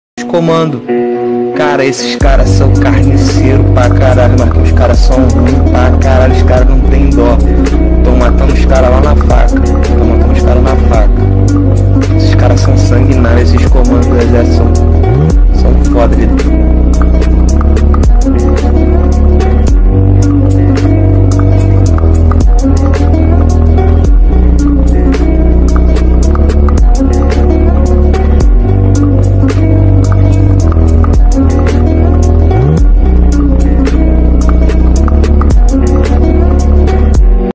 carioca: são carniceiro tão matando os cara na faca | cleardo Pereira
audio de traficante falando: os comandos são carniceiro tão matando os cara lá na faca
vídeo do exercito brasileiro para status e motivação